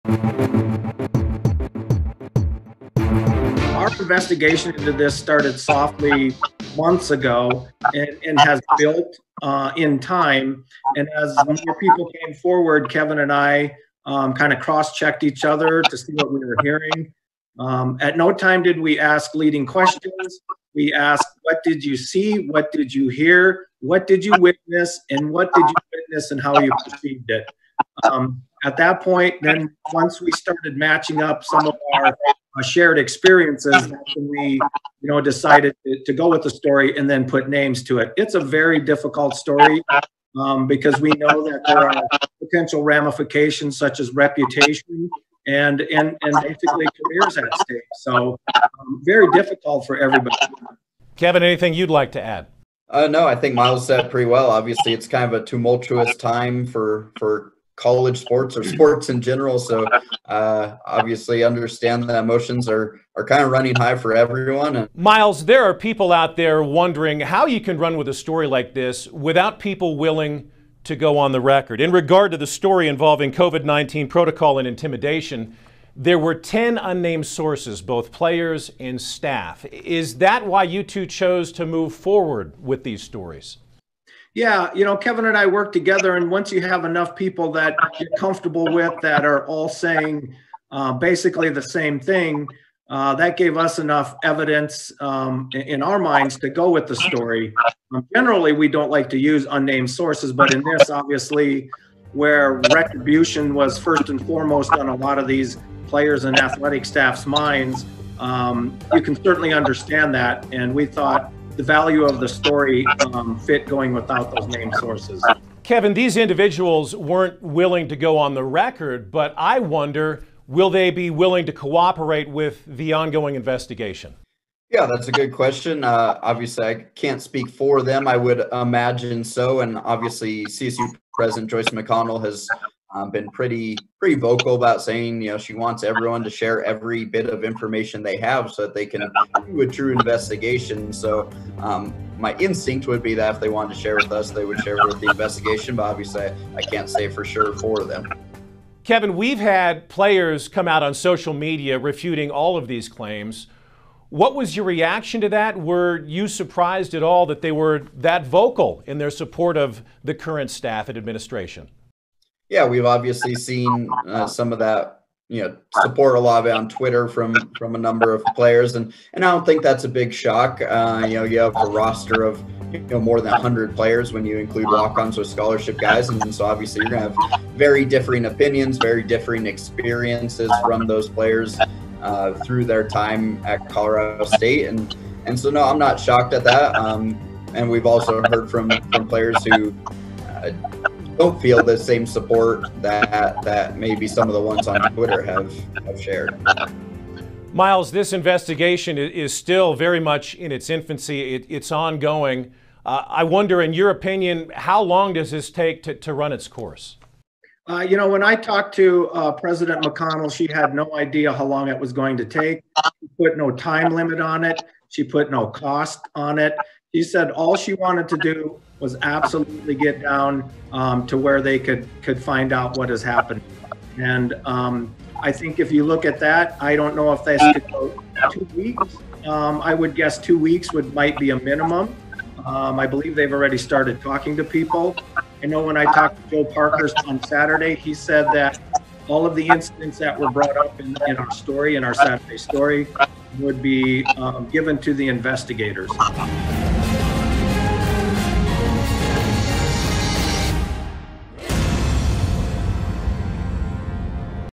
Coloradoan writers stand behind their reporting on CSU football
There have been allegations involving COVID-19 protocol and accusations of racial insensitivity and abusive behavior. The University has halted football activities.